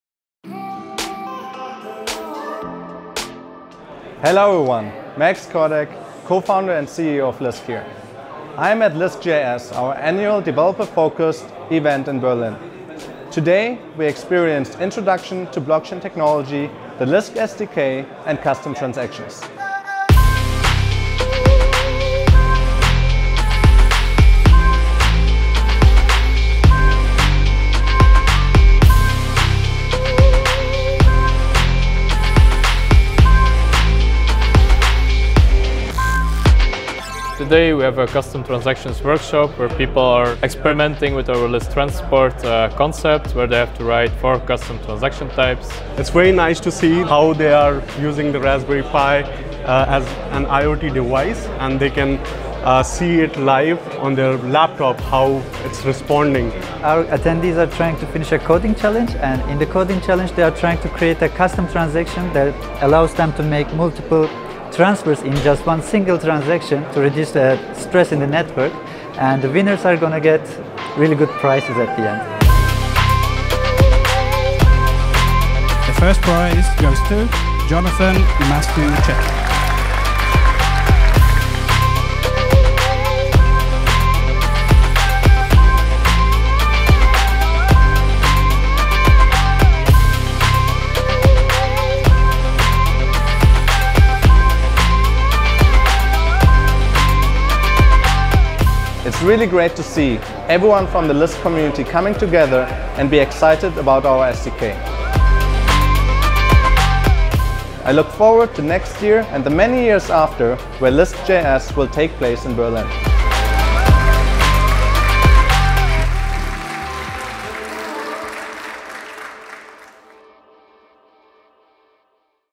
Lisk.js 2019 | Developer Event in Berlin | Aftermovie
On November 19th, we hosted our annual blockchain event in Berlin. Thank you to everyone who was able to attend and see you all next year.

Developers learned how to code with Lisk’s SDK during hands on workshops, listened to keynotes from industry insiders and networked with local blockchain enthusiasts.